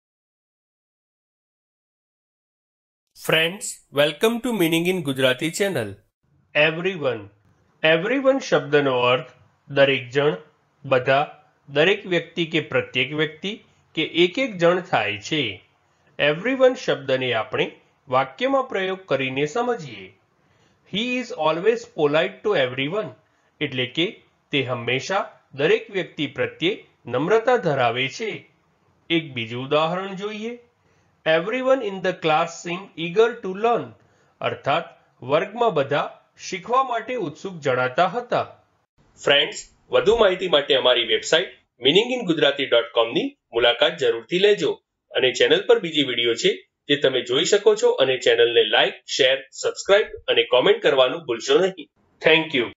Everyone Meaning in Gujarati | Everyone નો અર્થ શું છે | Everyone in Gujarati Dictionary
આ વિડિયો માં તમે Everyone નો Gujarati માં અર્થ સમજશો અને એની સાથે Everyone નું pronunciation પણ શિખશો. એટ્લે કે તમે Everyone ના meaning ની સાથે સાથે એ પણ શિખશો કે Everyone ને કેવી રીતે બોલાય, Everyone ને બોલવાની સાચી રીત કઈ છે.

તો બસ એક મિનટ માં શીખો Everyone શબ્દ ને.

Everyone ને examples થી સમજીએ